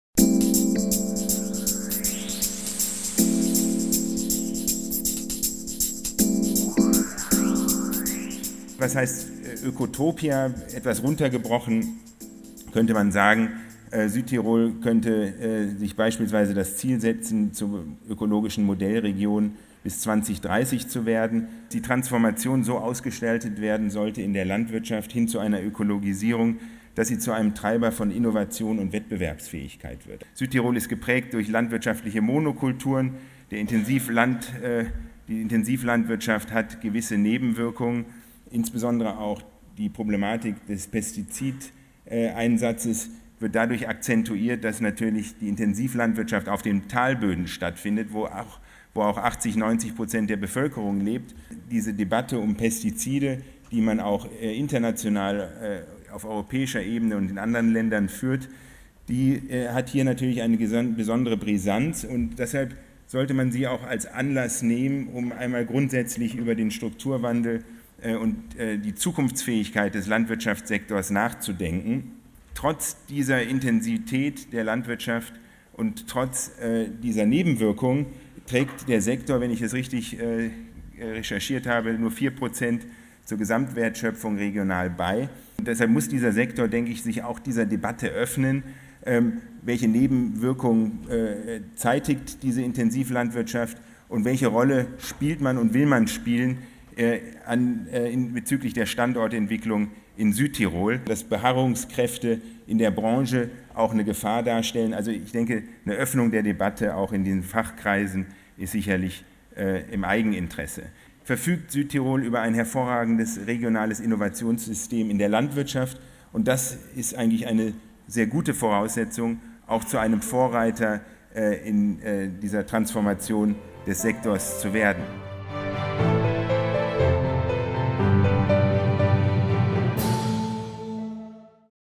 Trailer - Keynote Speech Daniel Müller-Jentsch I 9. Global Forum Südtirol 2017
9. Global Forum Südtirol 2017 I Daniel Müller-Jentsch: Ökotopia Südtirol 2030 - Nachhaltige Regionalentwicklung aus ökonomischer Perspektive I Ecotopia Alto Adige 2030 – Sviluppo regionale sostenibile in prospettiva economica

„Heutzutage gehört das Prinzip der Nachhaltigkeit zum gesellschaftlichen und wirtschaftlichen Mainstream. Ganze Branchen, allen voran die Stromwirtschaft und Automobilindustrie, zeigen uns auf, dass die etablierten Akteure gut beraten sind, sich rechtzeitig diesem Wandel zu stellen.“
„Il principio della sostenibilità è parte integrante di un mainstream sia a livello sociale che economico. Interi settori, come ad. es. il settore energetico o automobilistico, ci fanno intendere l' importanza di affrontare al più presto il processo di trasformazione“.

Ökonom, Avenir Suisse, Zürich (SUI) I Economista, Avenir Suisse, Zurigo (SUI)

Full Keynote Speech Daniel Müller-Jentsch